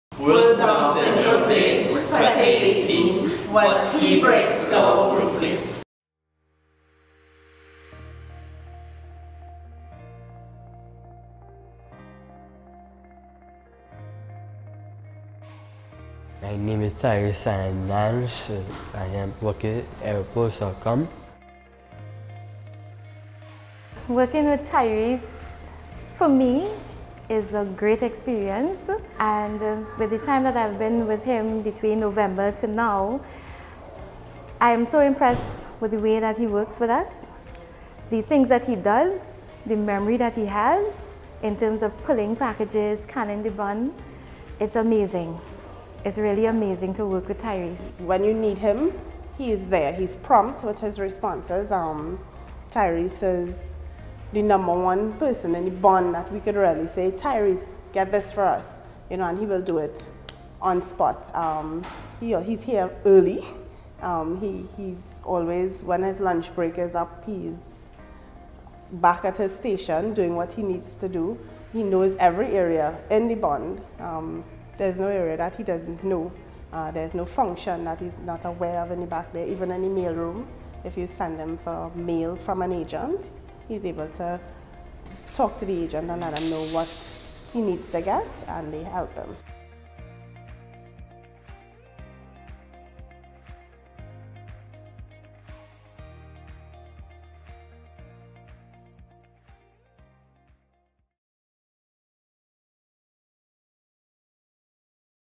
Trinidad DSFN WDSD 2018 Tyrese
The Down Syndrome Family Network (DSFN) in collaboration with Down Syndrome International (DSi) has featured one of our self advocates at his workplace for World Down Syndrome Day 2018